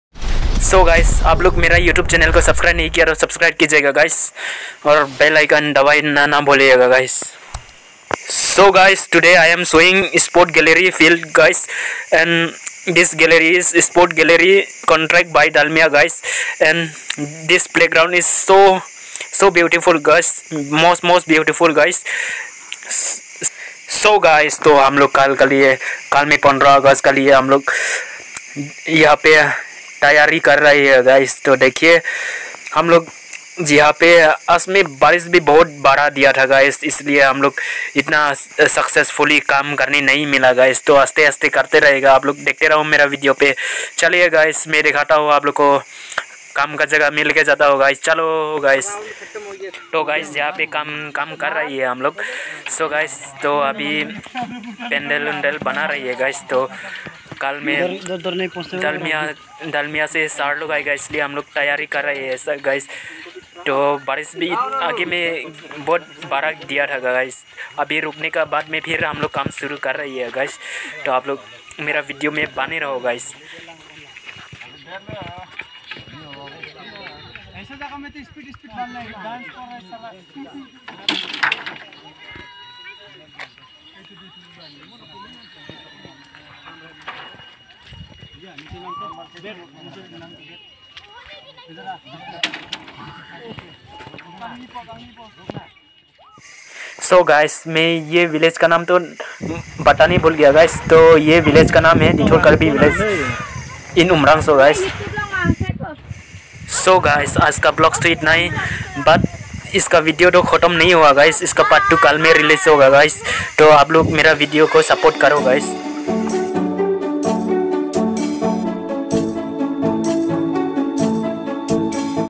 Title_Sports|| GALLERY Constructed By Dalmia CEMENT Bharat LTD CSR activities:Dithur karbi village
YEAR:2022-23 IN Umrongso District,

DIMAHASO.ASSAM

please 🙏 Mera Youtube channel's ko support karo guys

And advance, independence pendants day 🇮🇳🇮🇳🇮🇳🇮🇳🇮🇳